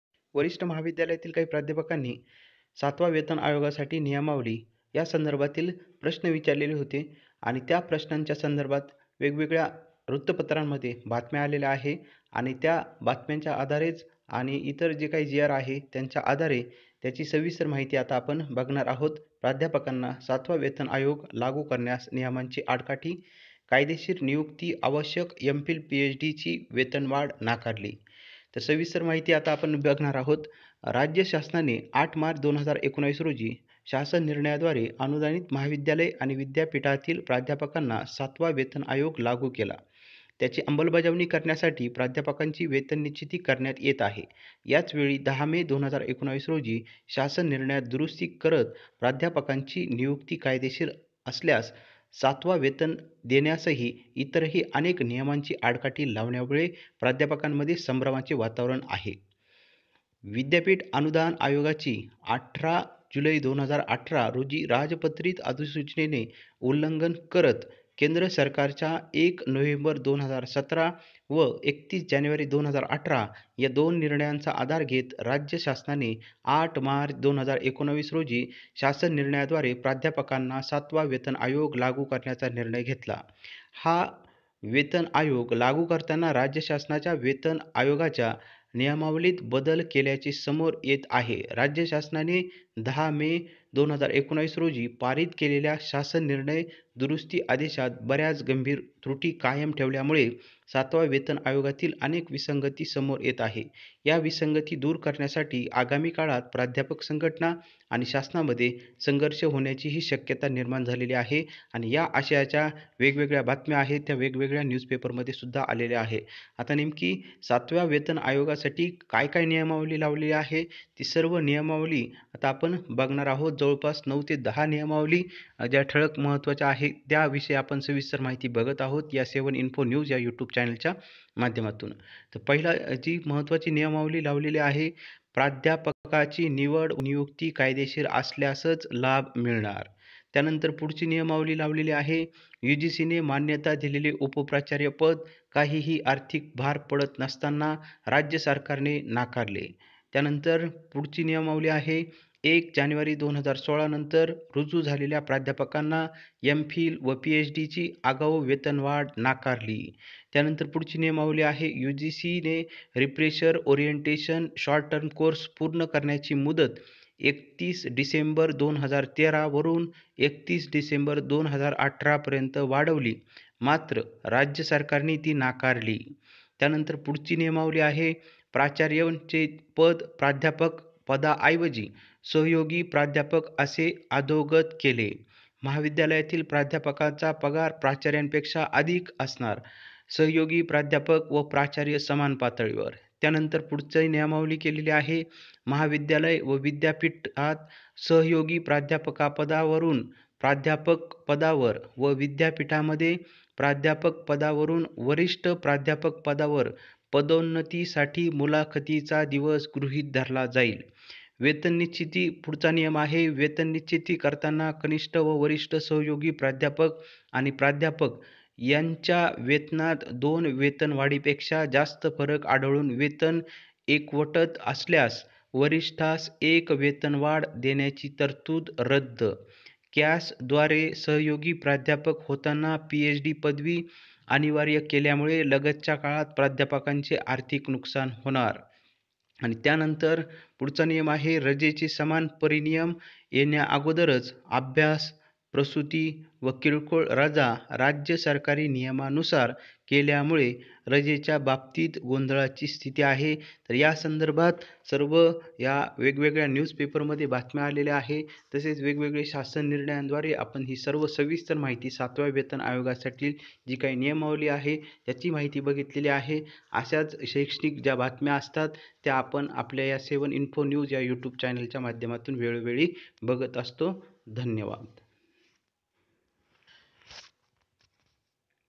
प्राध्यापकांना सातवा वेतन आयोग लागू करण्यास नियमांची आडकाठी
7 Pay Scale Professor, Assistant Professor Senior College, University. प्राध्यापकांना सातवा वेतन आयोग लागू करण्यास नियमांची आडकाठी